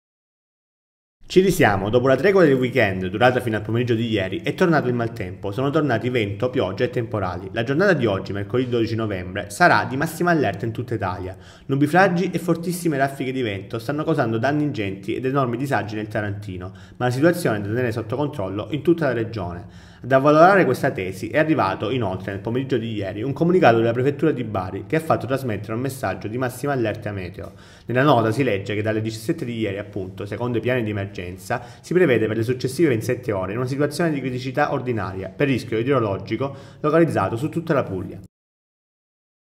Maltempo, disagi in tutta Italia. Massima allerta anche in Puglia
La giornata di oggi, mercoledì 12 novembre, è una giornata di massima allerta in tutta Italia. Nubifragi e fortissime raffiche di vento stanno causando danni ingenti ed enormi disagi nel tarantino, ma la situazione è da tenere sotto controllo in tutta la Regione. Ad avvalorare questa tesi è arrivato, inoltre, nel pomeriggio di ieri, un comunicato della Prefettura di Bari che ha fatto trasmettere un messaggio di massima allerta meteo.